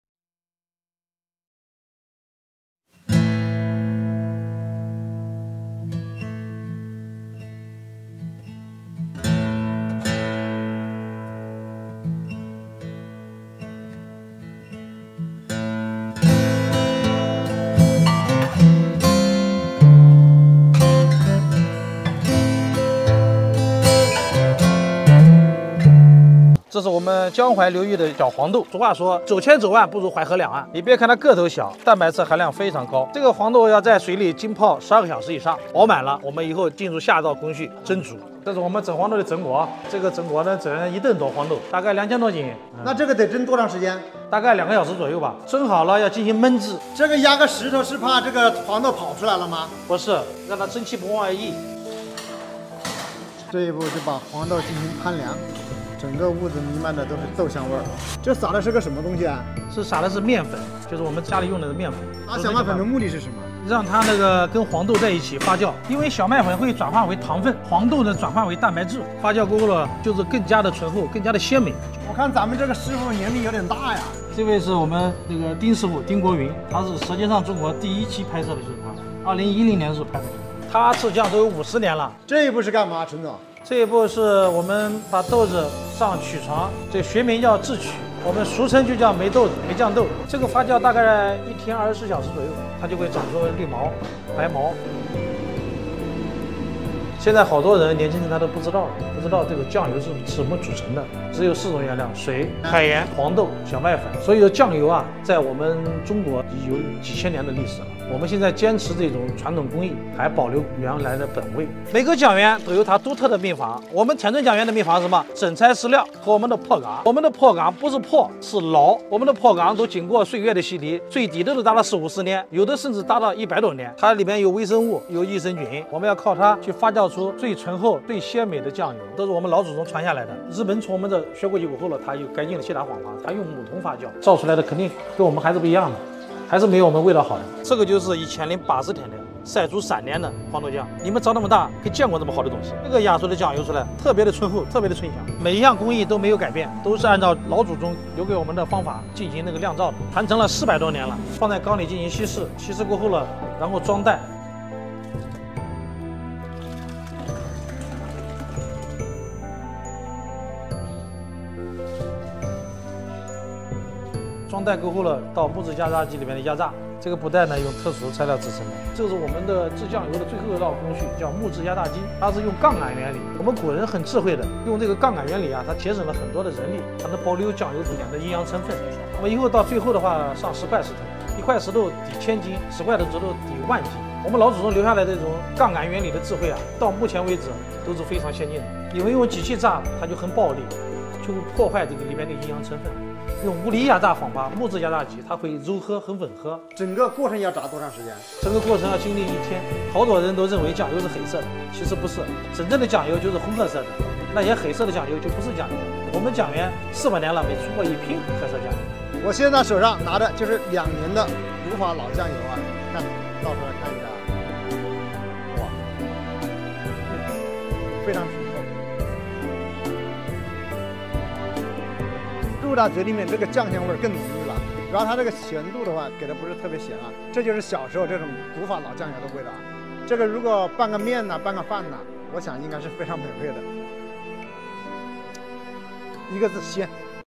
安徽大叔古法制酱油400年，3斤黄豆发酵3年才能出1斤酱油
大家好，我是【新哥走四方】
欢迎大家的点赞，评论，谢谢支持~